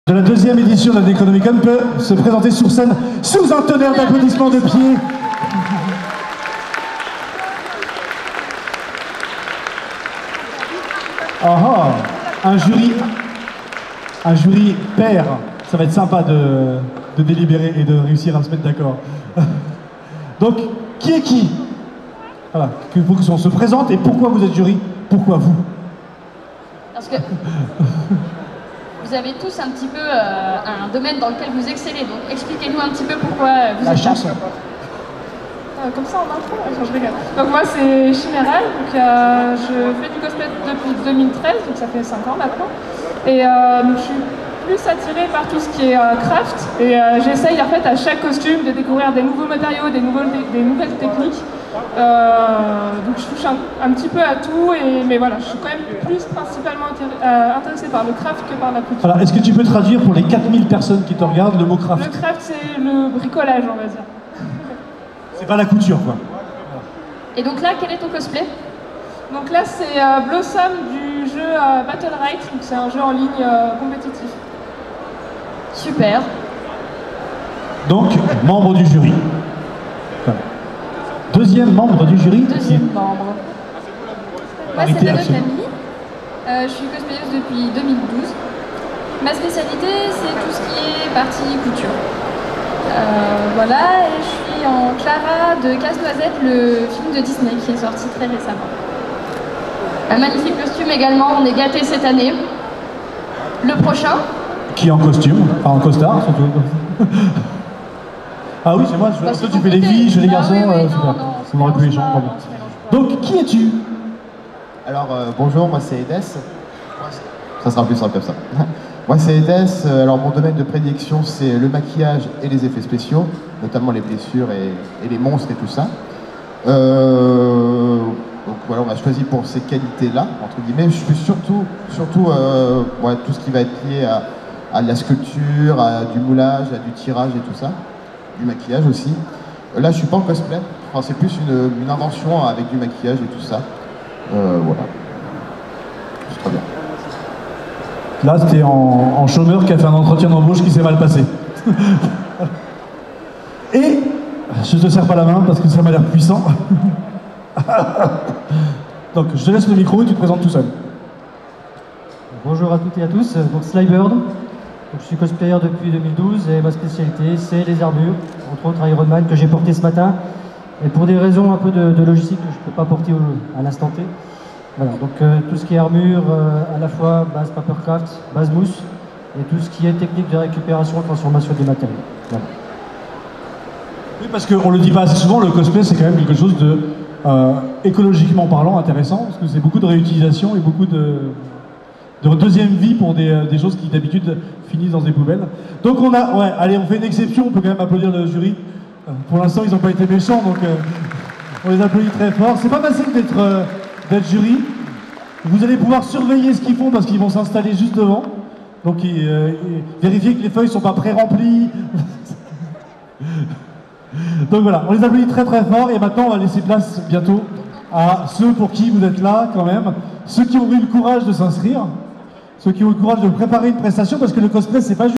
Necronomi'con 2019 - Introduction du jury CFC
Concours cosplay à Necronomi'con 2019 de Belfort (F)
Organisé par Cosplay.cow
Présenté par Elodie Toyac et  Fabrice Kakemono Camphrier
Jugés par Edes, Chimeral CosplayArt, Slybird&Marie Cosplay, Nanokami